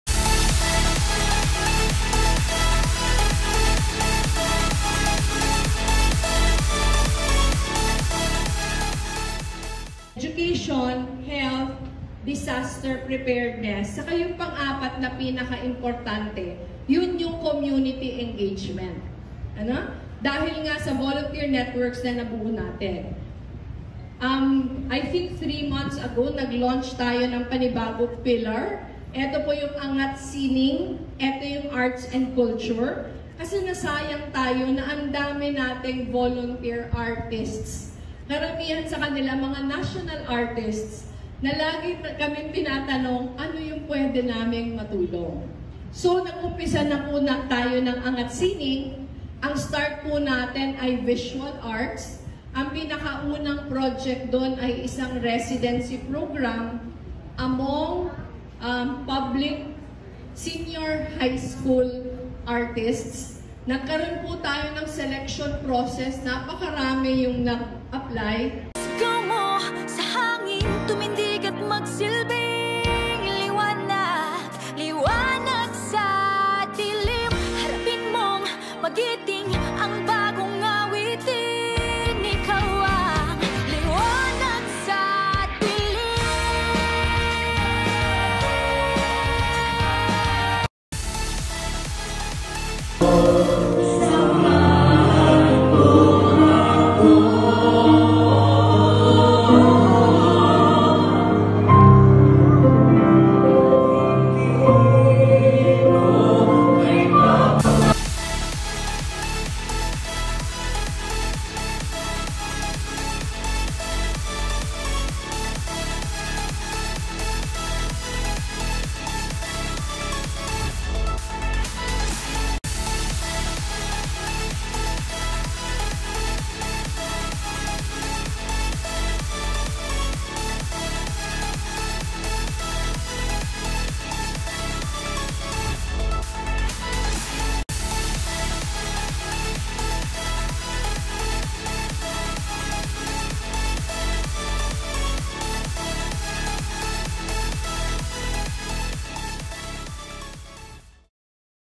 Tuloy Lang Cavite | Ang Pasasalamat with Atty. Leni Robredo
TuloyLang Cavite — Ang Pasasalamat with Atty. Leni Robredo at General Trias | 02 Dec 2023